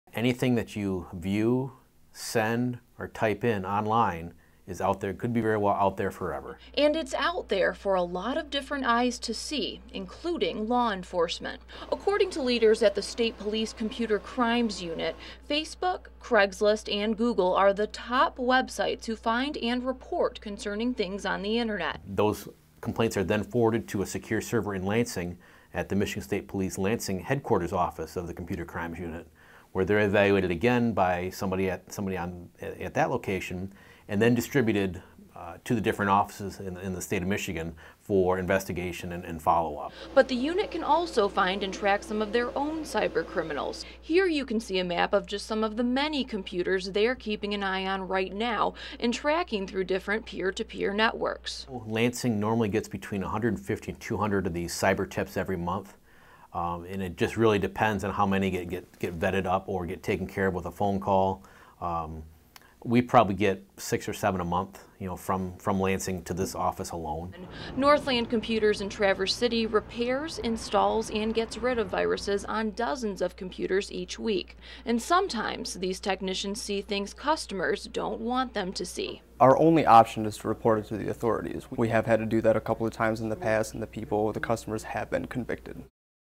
Investigators tackle computer crimes with help from others
An investigation into a Cadillac child porn case began when Google discovered pornographic pictures that were eventually forwarded to the Michigan State Police Computes Crimes Unit.  The CCU headquarters in Lansing receives hundreds of these kinds of tips each month that help put cyber criminals behind bars.